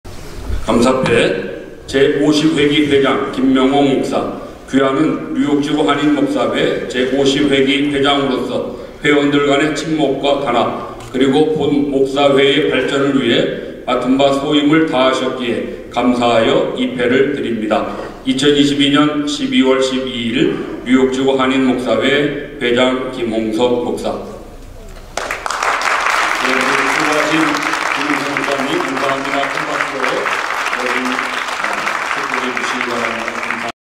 감사패 증정
뉴욕목사회 제51회기 이,취임식
2022년 12월 12일(월) 오후 5시 @하크네시야교회